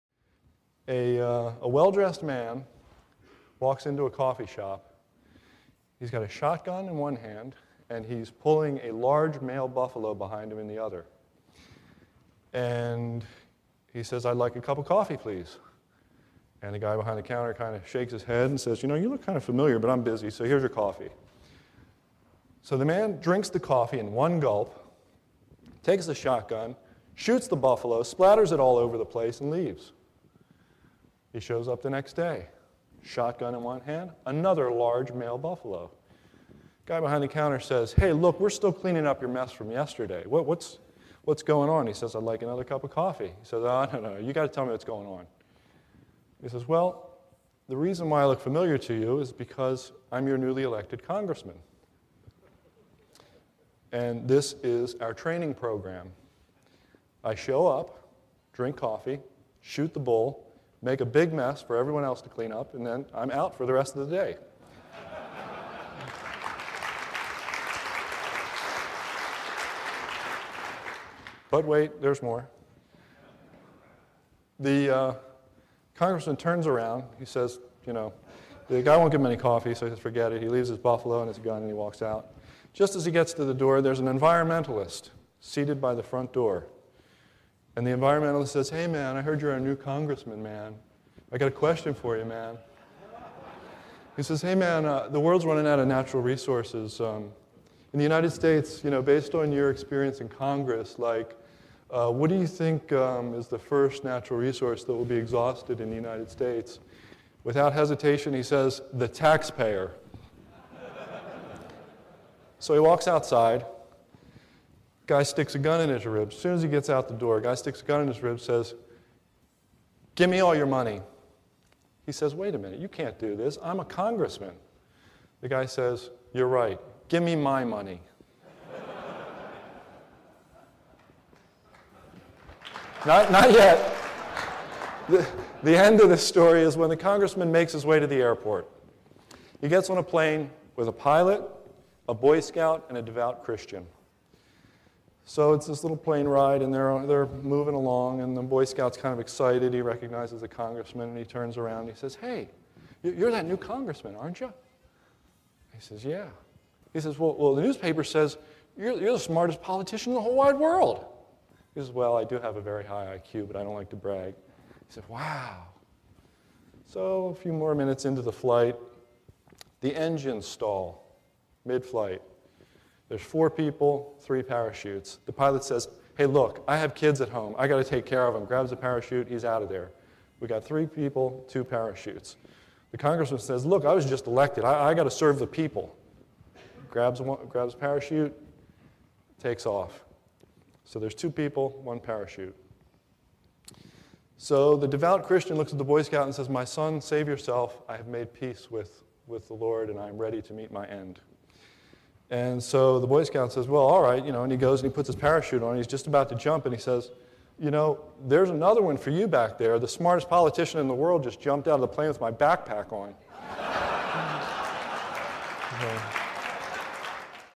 Congressman in a Coffee Shop
--- The smartest politician in the whole wide world explains his "training program" and much much more...